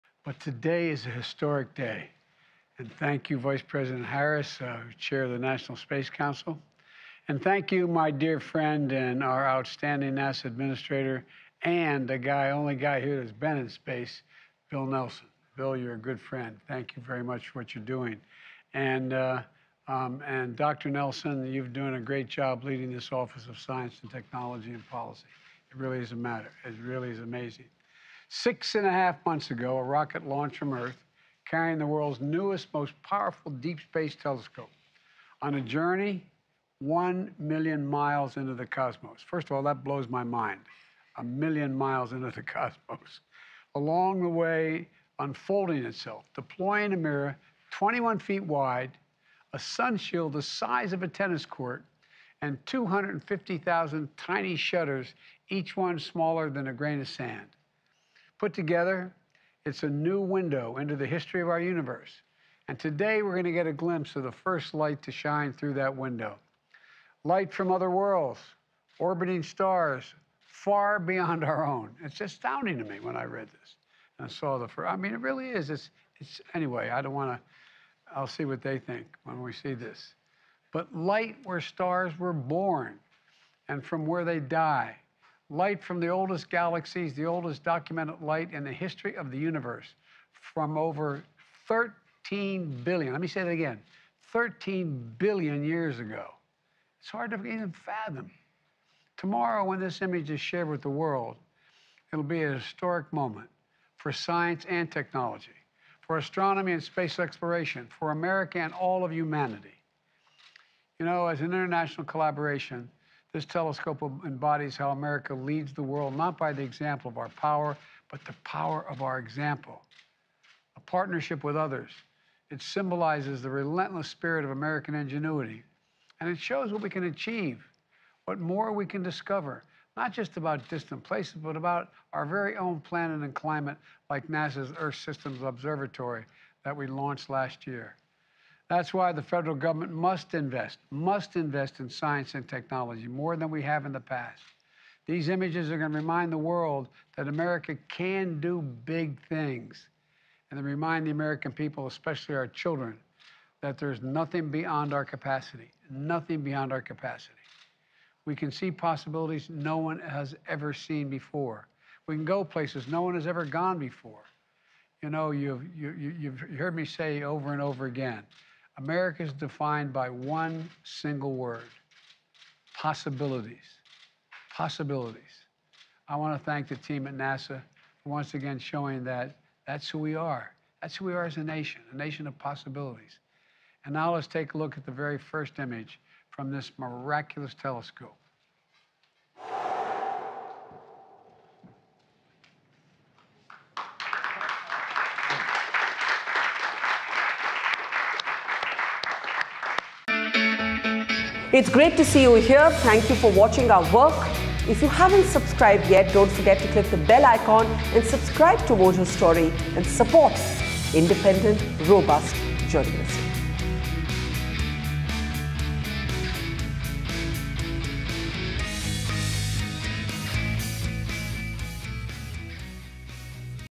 "Historic Moment" | Watch This Stunning Image of Galaxy As Prez Biden Unveils It To The World
Terming it a historic moment, US President Joe Biden unveiled the 'deepest and sharpest infrared image of the universe'. NASA's James Webb Telescope has produced the image. NASA said, "Thousands of galaxies – including the faintest objects ever observed in the infrared – have appeared in Webb’s view for the first time."

The image is one of the first color images by the telescope. "The image shows the galaxy cluster SMACS 0723 as it appeared 4.6 billion years ago. The combined mass of this galaxy cluster acts as a gravitational lens, magnifying much more distant galaxies behind it," NASA said in a statement.